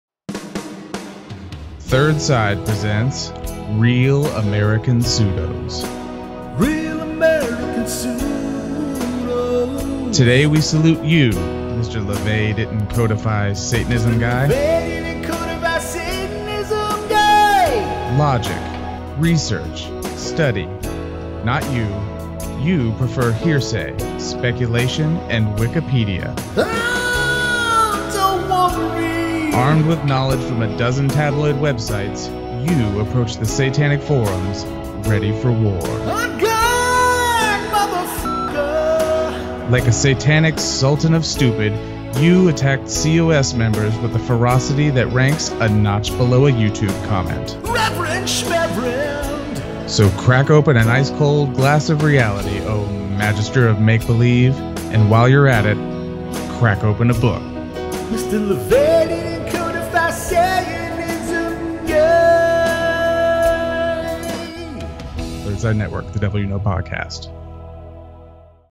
Real American Pseudos - Mr. LaVey Didn't Codify Satanism Guy - Third Side Network
A parody of the old Bud Light Commercials where we show our appreciation for Pseudos and thank them for their high level of stupidity.

Creator/Announcer: Jared McMammon
Vocals: John H Shaw
Writers: Magus Peter H Gilmore, John H Shaw & Jared McMammon

Edutainment Purposes Only